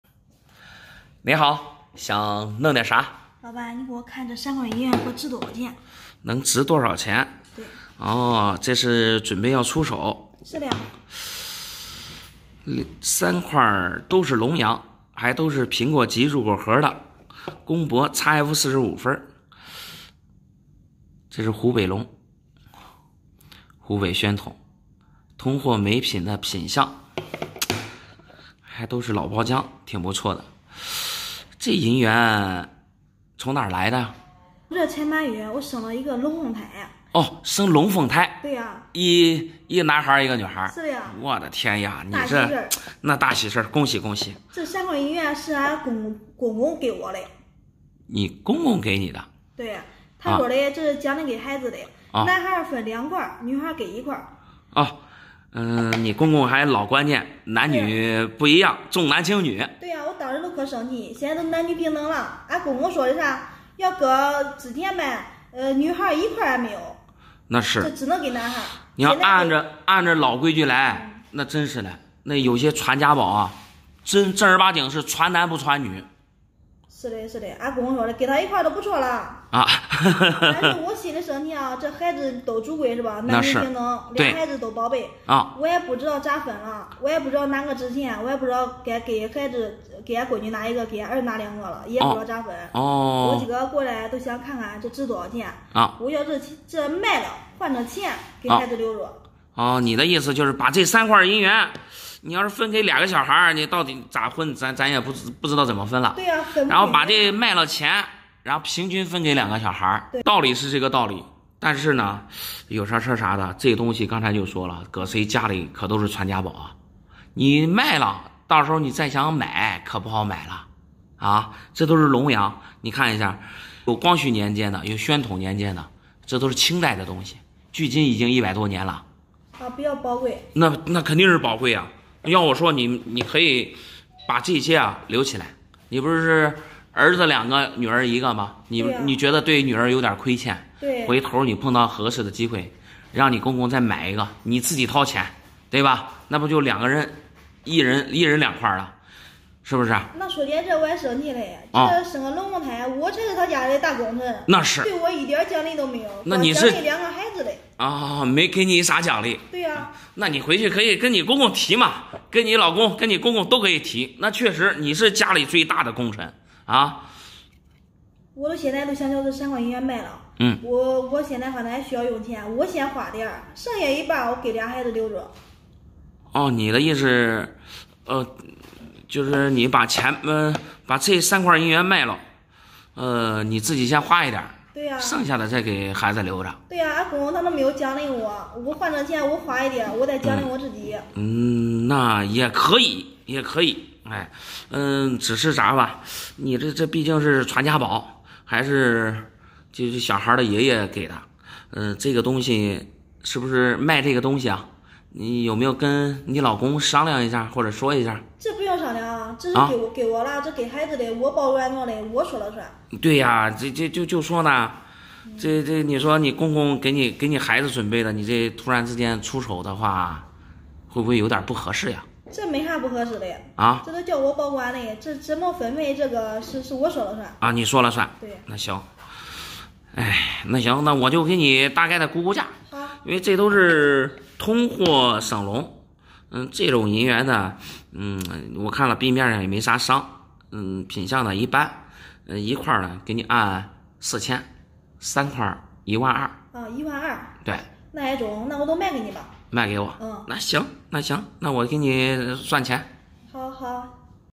生龙凤胎爷爷送银元当见面礼，因重男轻女惹出祸，儿媳这样做对吗【小闫打铁镯】
请放心，本视频是一部原创拍摄的视频，没有任何不良行为。 生龙凤胎爷爷送银元当见面礼，因重男轻女惹出祸，儿媳这样做对吗